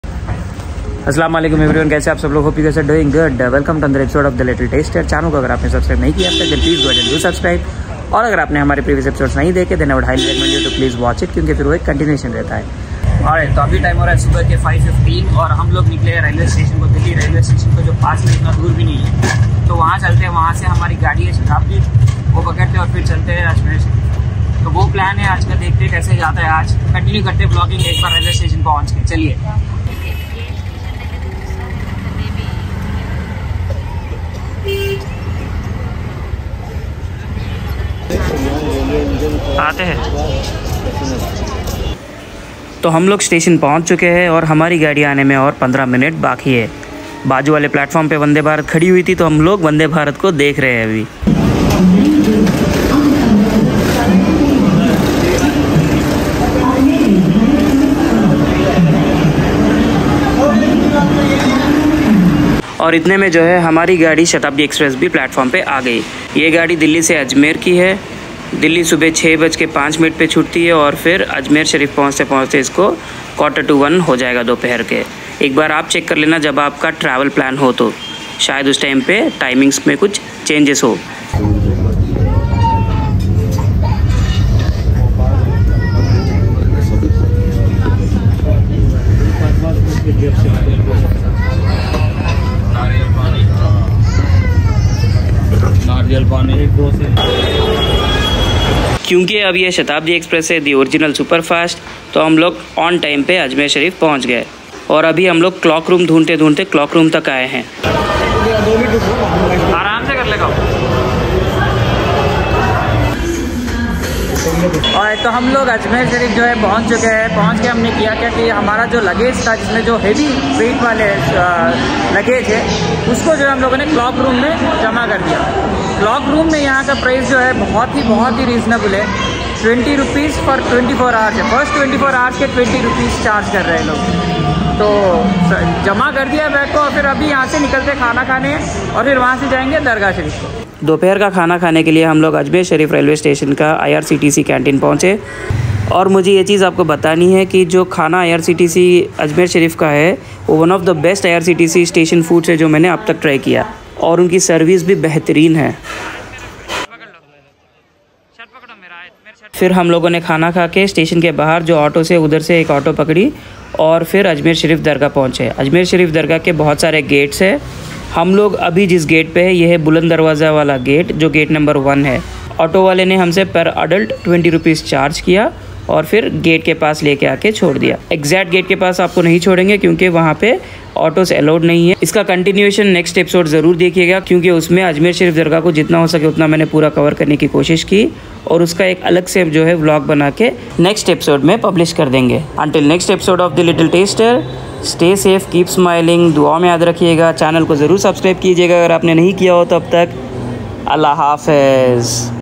LEAVING DELHI RAILWAY STATION WITH AMAZING EXPERIENCE | DELHI TO AJMER TRAIN | SHATABDHI EXPRESS
LEAVING DELHI WITH AMAZING EXPERIENCE |  DELHI TO AJMER TRAIN | SHATABDHI EXPRESS